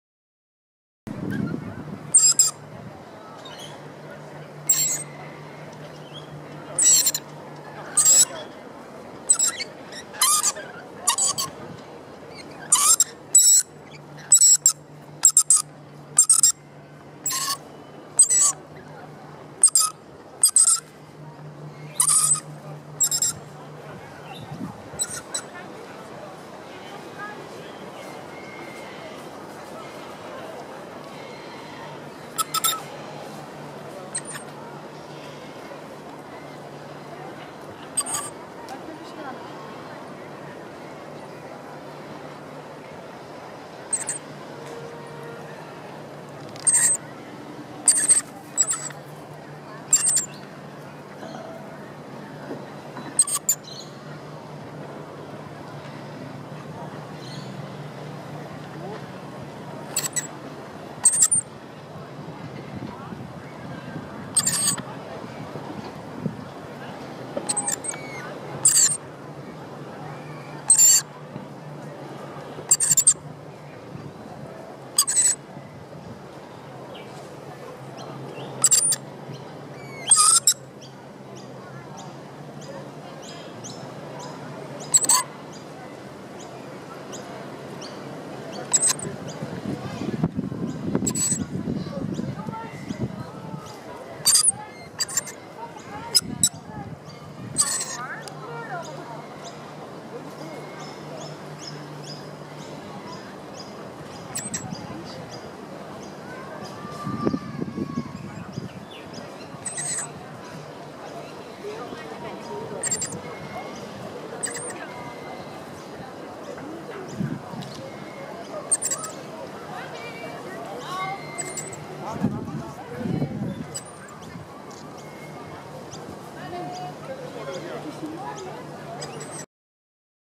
Rainbow Lorikeet @Royal Botanical Gardens Sydney
Rainbow Lorikeet with Opera House in background.